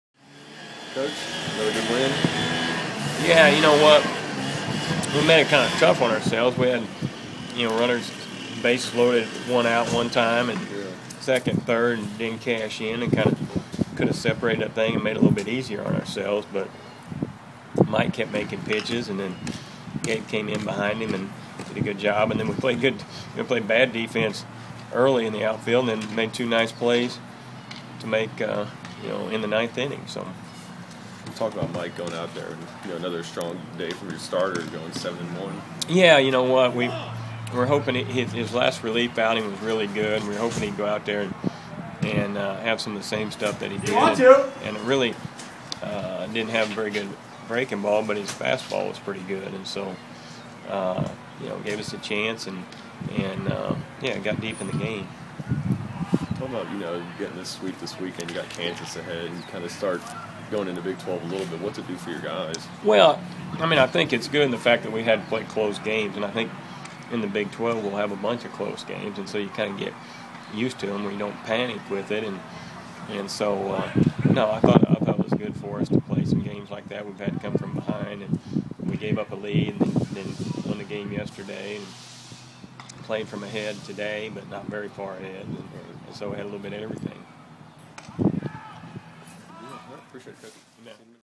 Frank Anderson Interview - Cowboy Baseball vs. New Mexico
Oklahoma State head coach Frank Anderson speaks after the Cowboys' 2-0 win over the New Mexico Lobos. The Cowboys swept the series with UNM, and improved to 12-3 on the 2011 campaign. OSU hosts Central Arkansas on Tuesday at Allie P. Reynolds Stadium before opening Big 12 play at Kansas.